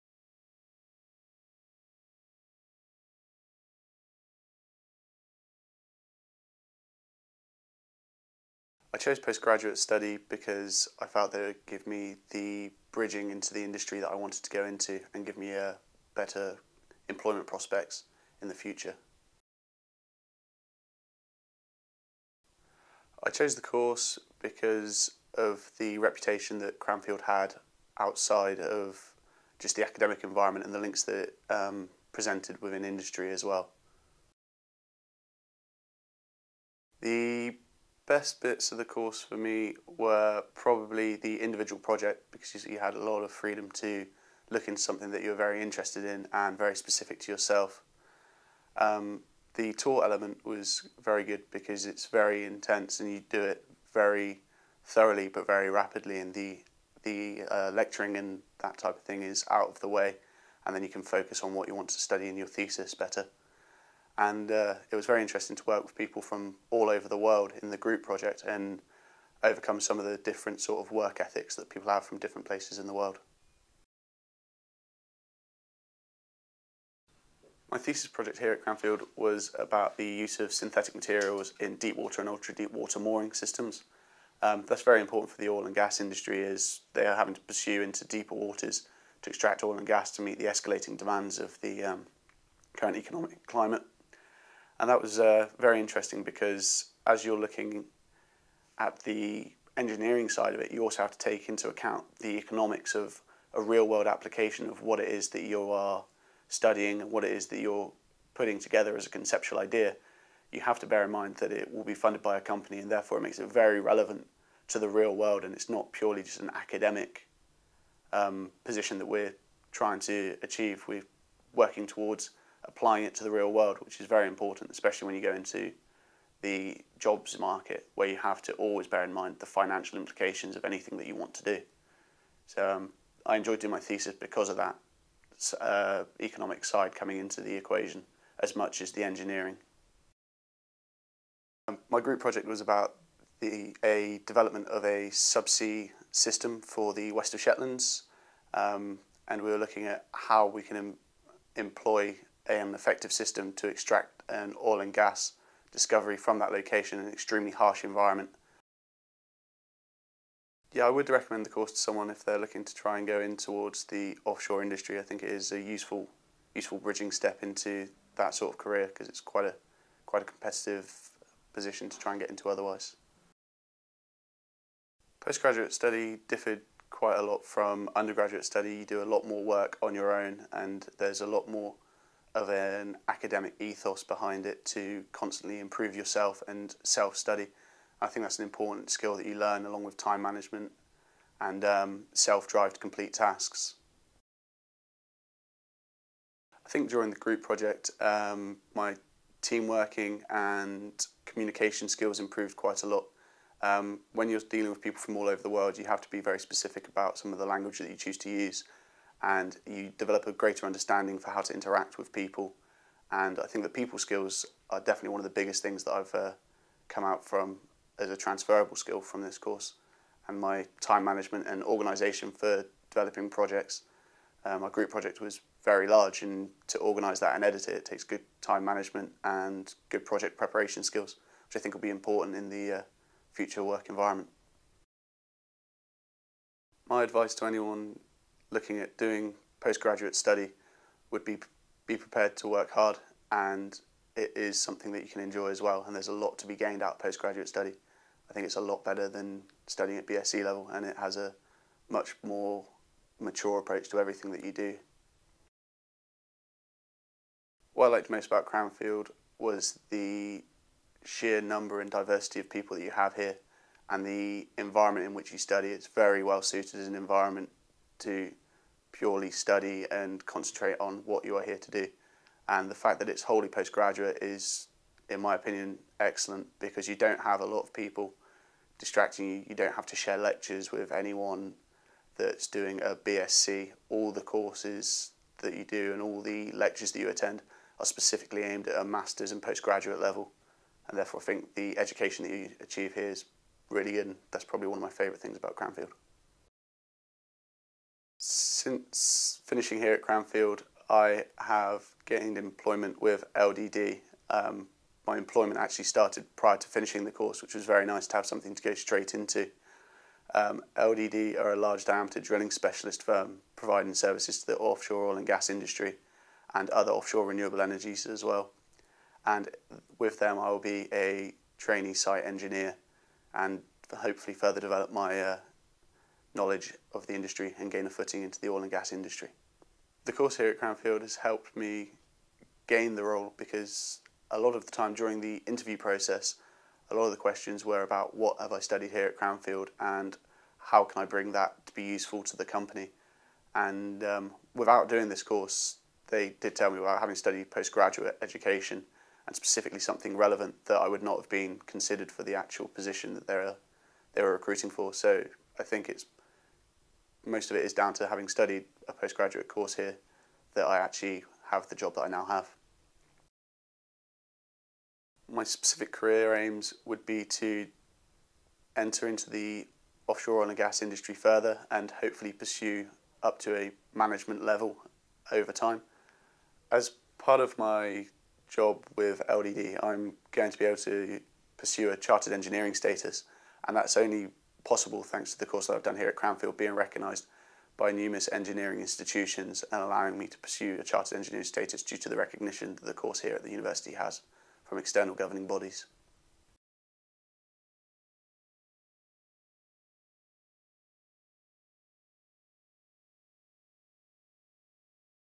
Offshore and Ocean Technology MSc at Cranfield University
Offshore and Ocean Technology MSc at Cranfield University is a full and part time accredited course.